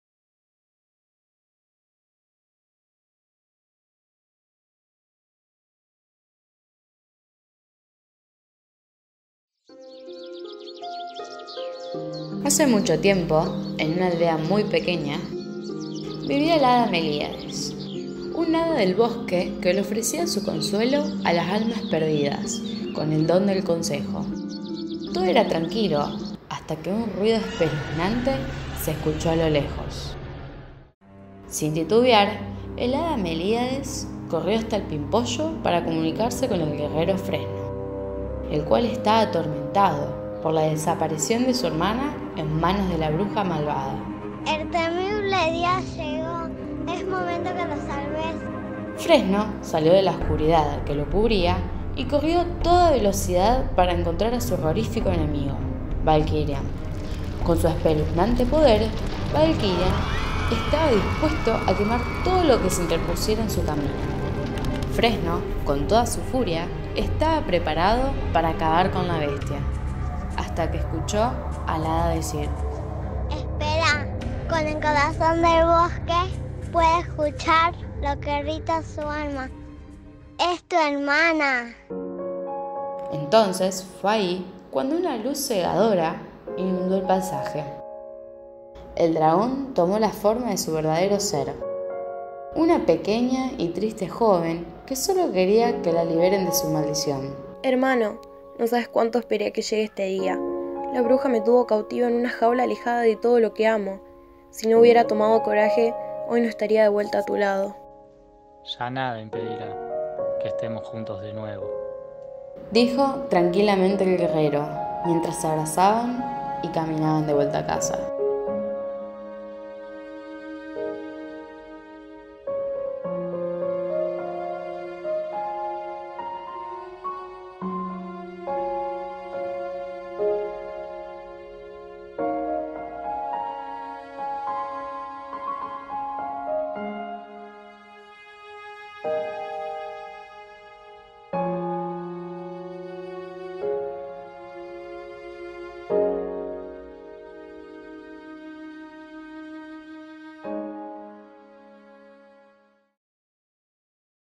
"En el interior de los árboles", proyecto de tesis en fotografía, de Milagros Vega
He aquí mi proyecto de tesis; un proyecto en el cual decidí sobrepasarme de mis capacidades para ver qué tan buena puedo ser si me lo propongo, donde puedo ver que mi capacidad no tiene límite, no hay un techo que me detenga. Elegí hacer esto porque soy fanática de los cuentos de hadas y de Photoshop, ¿qué mejor combinación que esa? No me queda más que agradecerles a todos, a mi familia por apoyarme siempre, a mis profesores por enseñarme tanto, en especial a mi mamá, Nacho, Delfi, Alma y Agus, por ayudarme a que esto salga espléndido, también a Marce por ser mi tutor, sos mi modelo a seguir como fotógrafo y fue un gran placer que me hayas ayudado.

El cuento, vídeo e imágenes son de mi autoría, a excepción Neuwaldegg 3 (foto de un bosque) de fallen-cherubim
 (extraído de deviantArt) y la imagen de fuego de Benjamín Lizardo (extraído de unsplash)